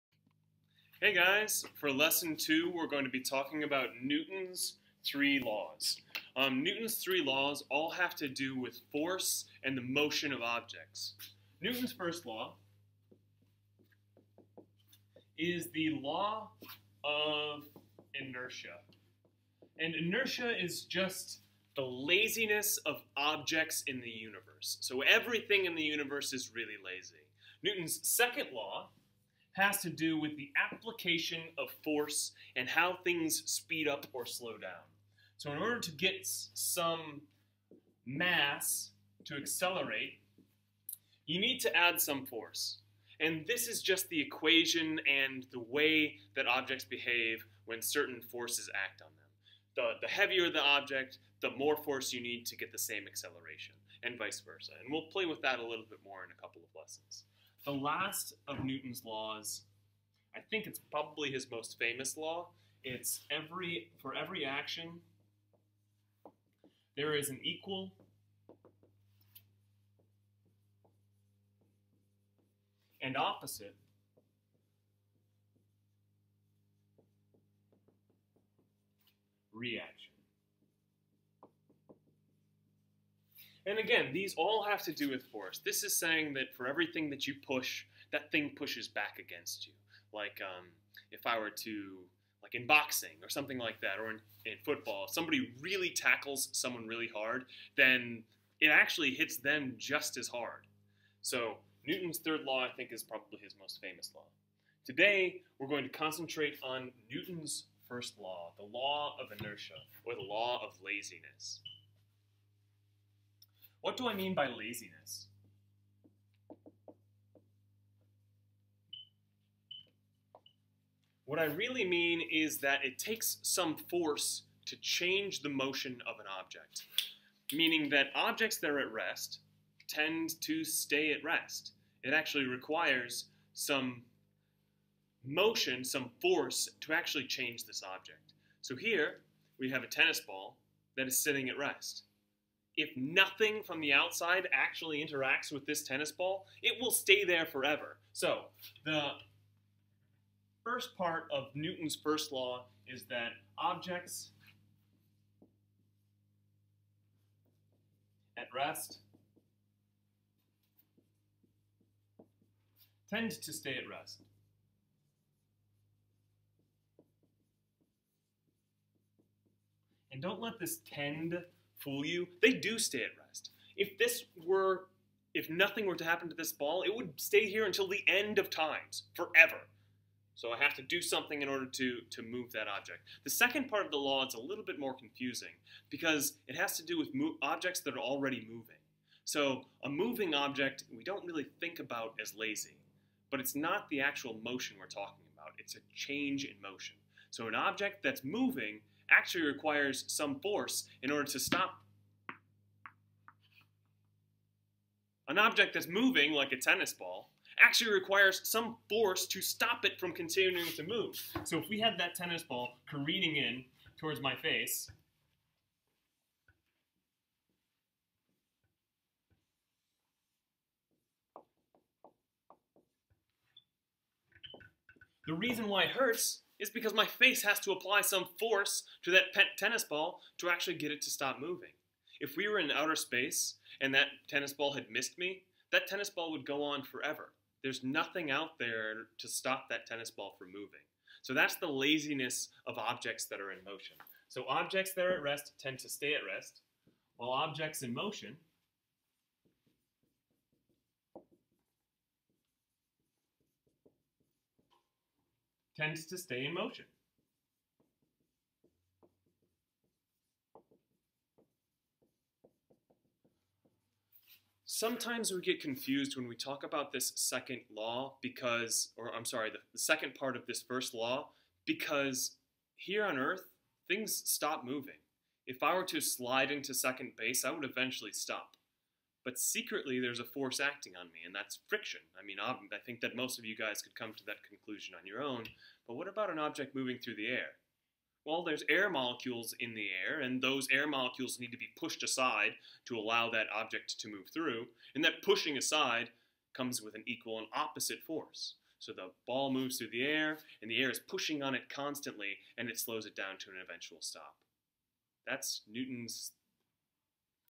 2015 - 02 Newton
For the second lesson we will discuss Newton's 3 laws of motion.